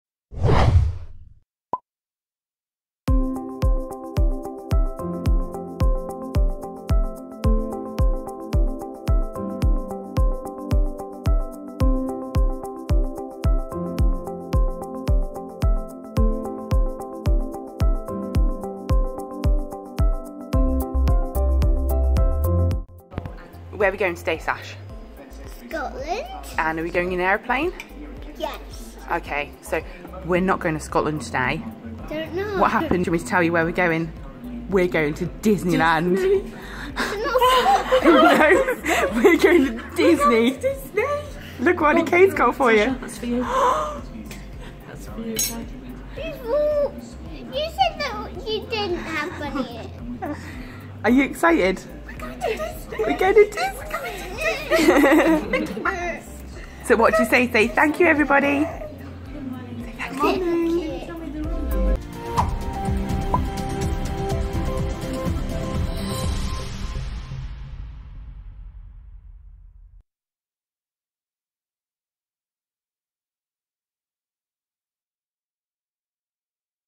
The moment hero six-year-old Sasha Nutley is told she is going to Disney World
Six-year-old Sasha Nutley, from Norwich, was rewarded a holiday when she won a special recognition award at the Stars of Norfolk and Waveney Awards in December 2015.
Earlier that year, when she was just four, Sasha saved her mum's life.
She dialled 999 after she was unable to wake her mum up when she fell unconscious from a heart condition.
For her bravery, she thought she was going to Scotland but was surprised to find she was going far, far away.